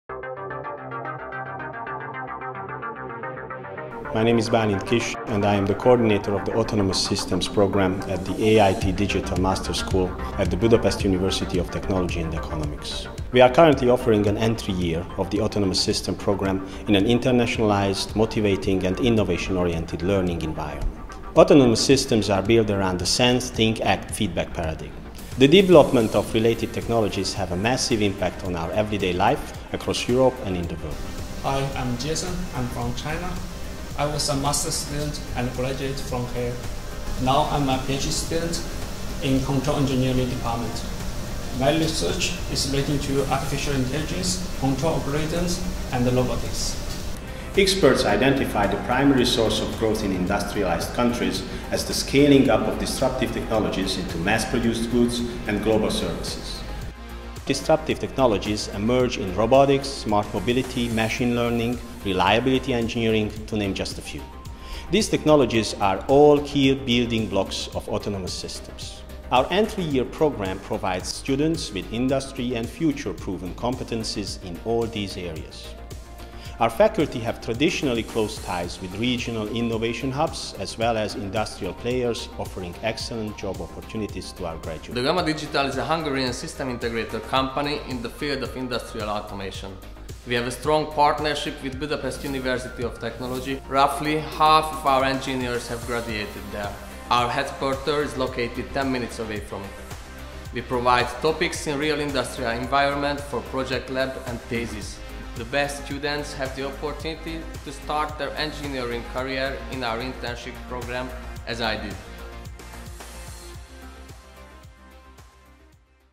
Autonomous Systems - EIT Digital MSc Programme
The EIT Digital Master School offers a two-year education
programme in which the two years (entry, exit) are completed in two
different European countries, and the two universities provide double
degrees combining technical competence with skills in Innovation and
Entrepreneurship.

BME-VIK offers Autonomous Systems entry year program with a didactic
balance between courses related to the reliable hardware and software
framework of autonomous systems and courses comprising knowledge to
design and robustly implement autonomous functionalities. The scope is
on integrating hardware and software into safe and secure embedded
systems that can work autonomously with high reliability and be applied
under critical requirements, e.g., in real-time and medical
applications.